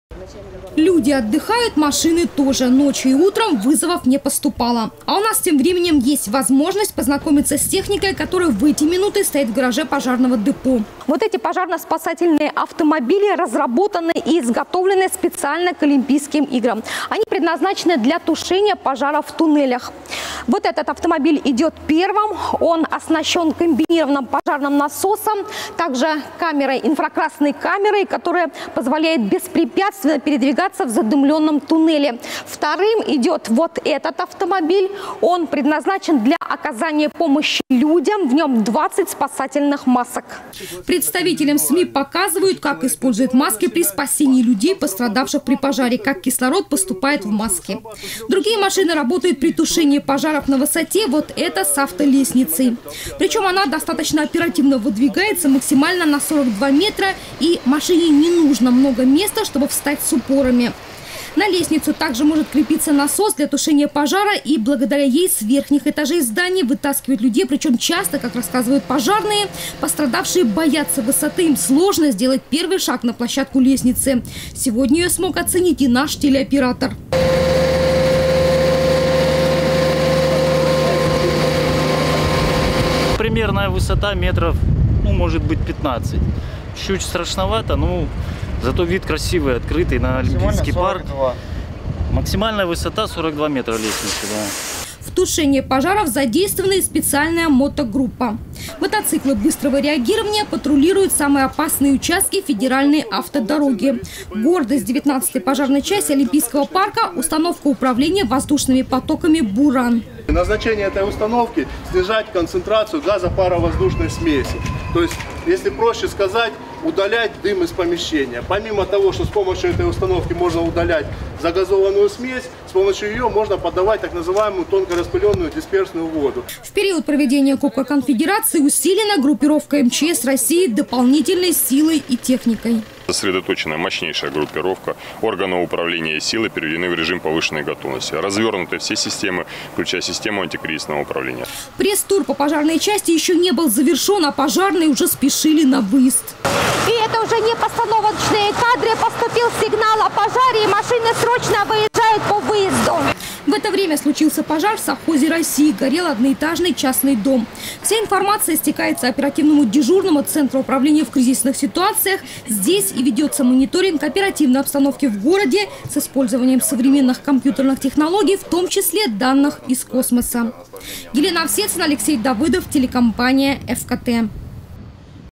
Современная пожарная техника задействована на Кубок Конфедераций в Сочи. Новости Сочи Эфкате
В проведении Кубка Конфедераций задействовано 720 казаков. Они охраняют общественный порядок вместе с полицией. Повышены меры безопасности на границе. Также усилена группировка МЧС России при проведении пожарной безопасности.
Сочи. Новости.
Телеканал Эфкате.